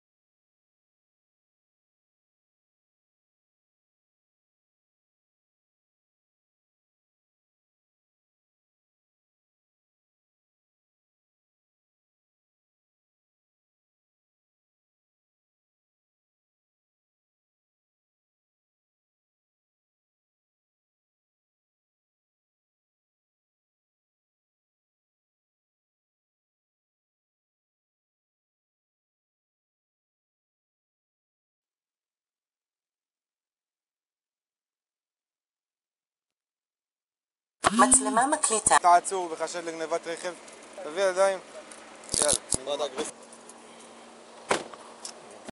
בפעילות מהירה של שוטרי מחוז צפון נעצר חשוד דקות לאחר שגנב רכב מתחנת דלק בעיר עפולה
בתאריך 20/06/24 התקבל דיווח במוקד 100 של משטרת ישראל אודות גניבת רכב מתחנת דלק בעיר עפולה.

למקום הוזנקו כוחות משטרה רבים שהחלו בפעולות לאיתור הרכב ובפעילות מהירה של שוטרי הסיור, זוהה הרכב בתנועה בכפר תבור נהוג על ידי החשוד.

מחומר החקירה עולה כי הקורבן השאירה את רכבה פתוח ומונע וירדה לחנות הנוחות בתחנת הדלק ואז החשוד שזיהה את הרכב פתוח נכנס לרכבה ונמלט במהירות מהמקום.

כאמור, החשוד תושב קרית מלאכי בן 30 נעצר ובהתאם לבקשת המשטרה מעצרו הוארך בבית המשפט השלום בנצרת עד לתאריך 28/06/24.

 משטרת ישראל קוראת לציבור הנהגים לוודא נעילת הרכב ברגע שיוצאים ממנו גם למספר דקות ,להשאיר את מפתחות הרכב בידיכם ולא להשאירו מונע ללא השגחה דבר שיכול למנוע את גניבתו או גניבת חפצים ממנו.

קרדיט: דוברות המשטרה